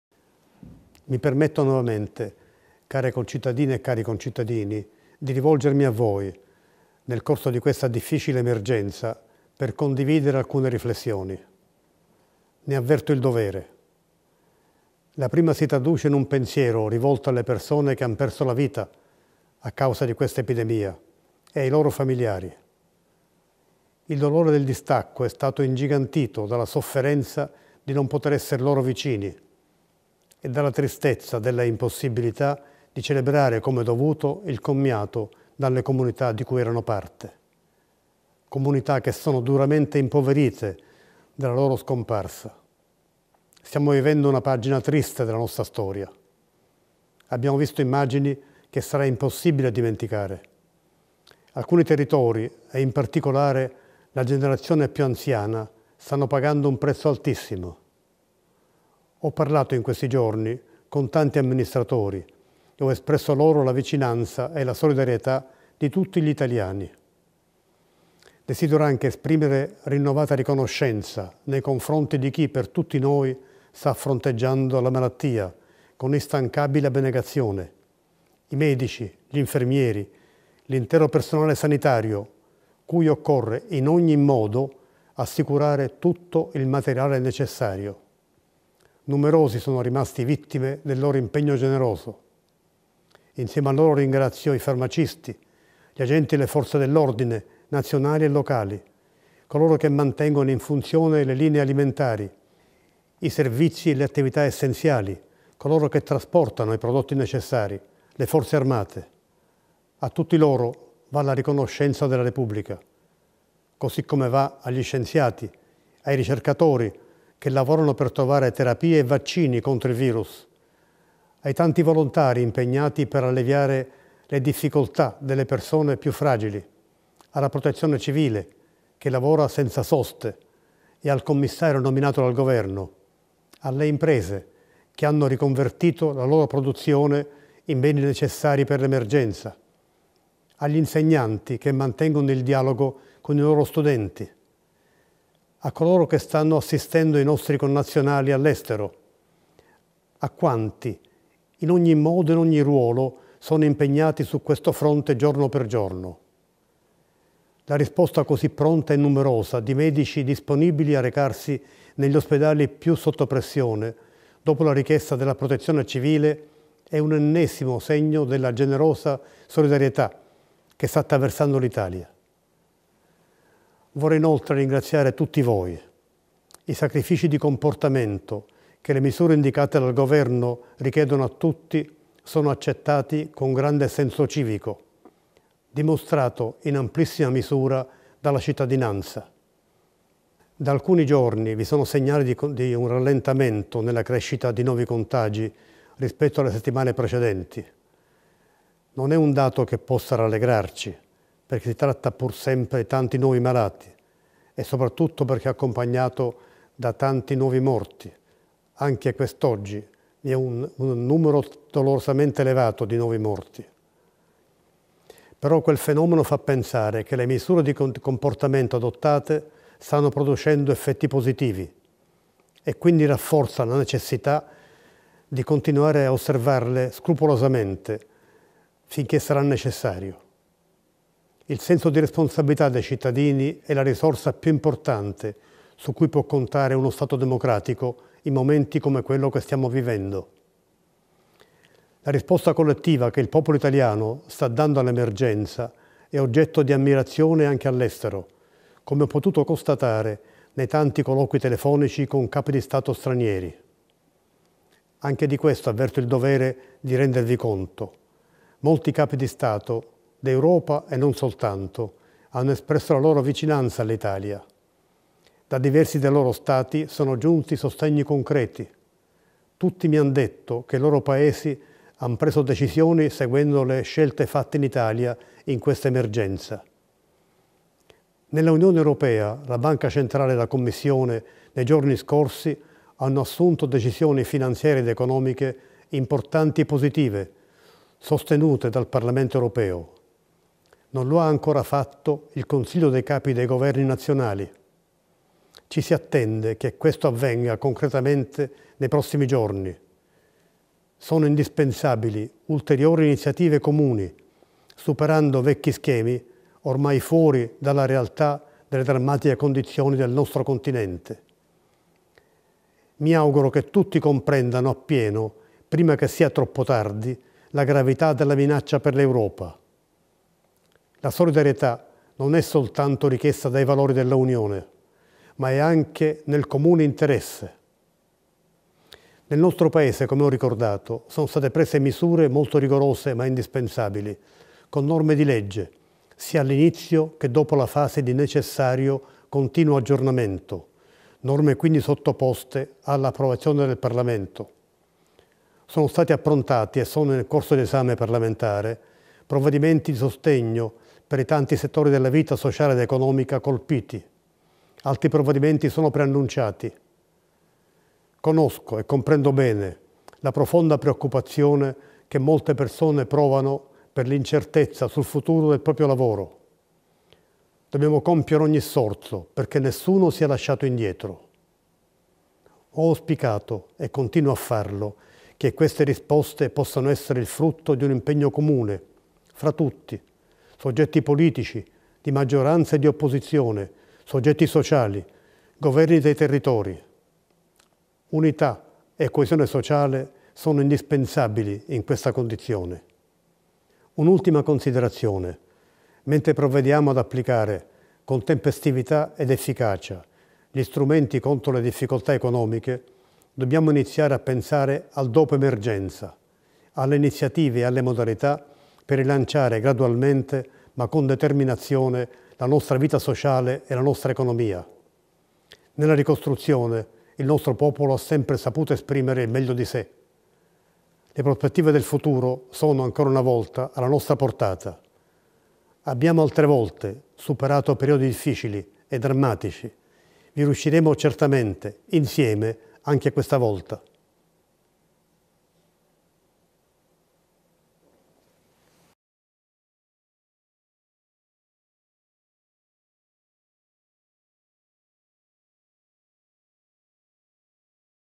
Dichiarazione del Presidente della Repubblica sull'emergenza coronavirus
Il discorso del Presidente della Repubblica: "L'Europa si renda conto della minaccia, prima che sia troppo tardi. Nostro senso di responsabilità ammirato in tutto il mondo"